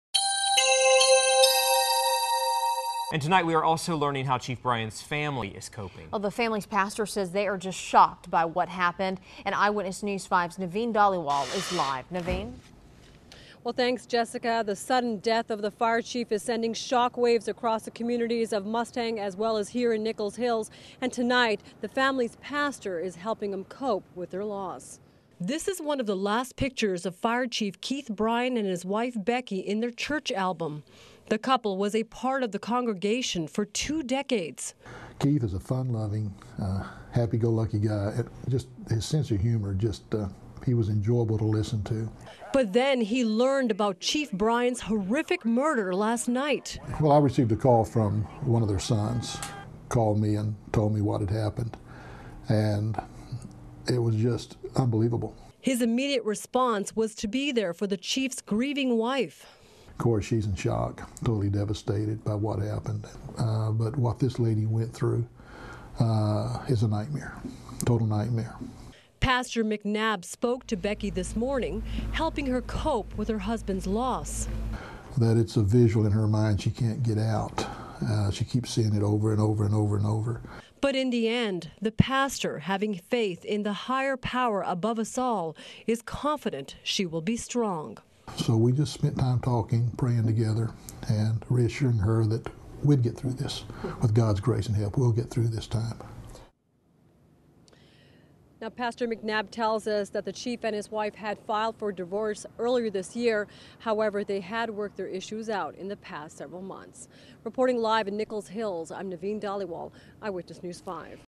Pastor Of Slain Fire Chief Speaks
Pastor Jim McNabb says Chief Keith Bryan's family is shocked by what happened.